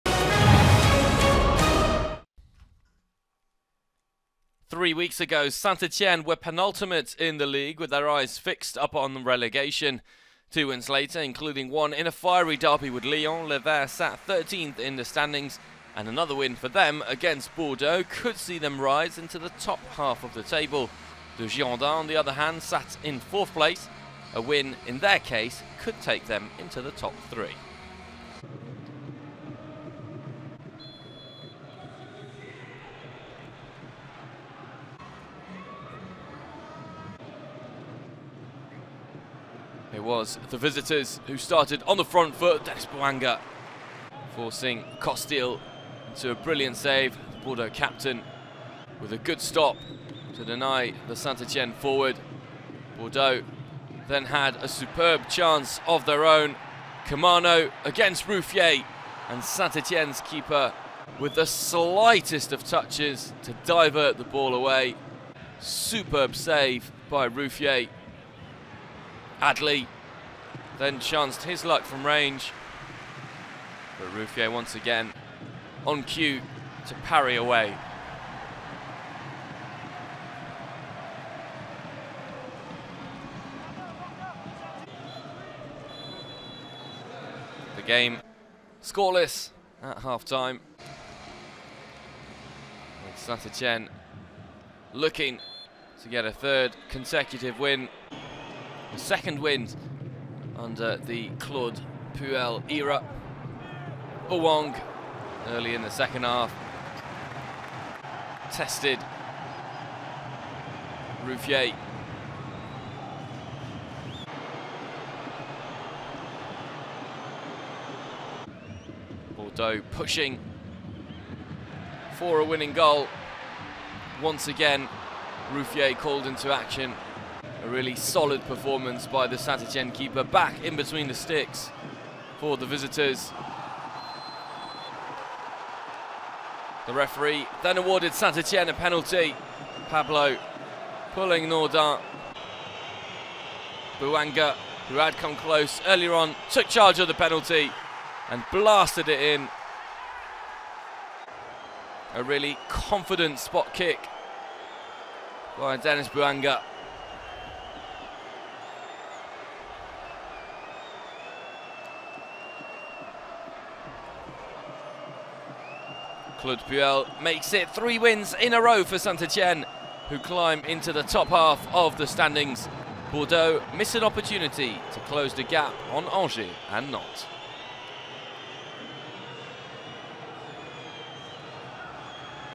Girondins de Bordeaux - AS Saint-Etienne ( 0-1 ) - Highlights - (GdB - ASSE) / 2019-20
Girondins de Bordeaux vs AS Saint-Etienne Highlights (0-1) in video.

Ligue 1 Conforama - Season 2019/2020 - Week 10 Matmut Atlantique - Sun, 20 Oct 2019

Goals : Denis BOUANGA (90' +2 pen - AS Saint-Etienne)

AS Saint-Etienne Team line-up : 4 - William SALIBA / 6 - Yann M'VILA / 7 - Ryad BOUDEBOUZ / 10 - Wahbi KHAZRI / 16 - Stéphane RUFFIER / 20 - Denis BOUANGA / 23 - Sergi PALENCIA / 24 - Loïc PERRIN (c) / 28 - Zaydou YOUSSOUF / 31 - Charles ABI / 32 - Wesley FOFANA